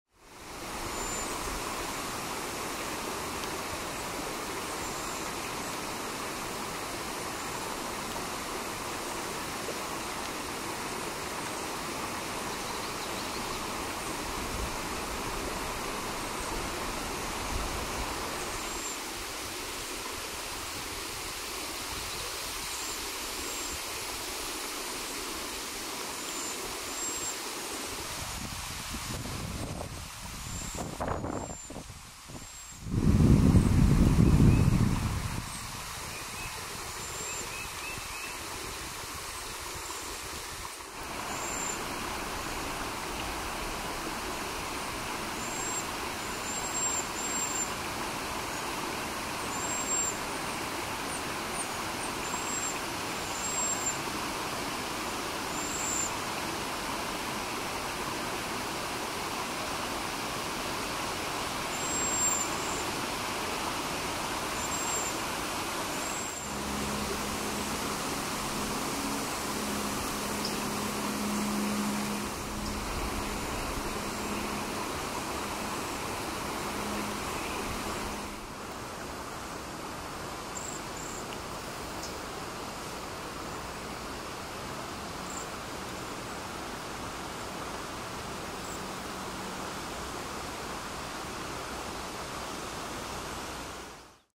20240629 cedar waxwings gay city state park. these borbs were remarkably friendly? or hostile?
One of them flew right up to me (either to say hi or to tell me to get out of the neighborhood).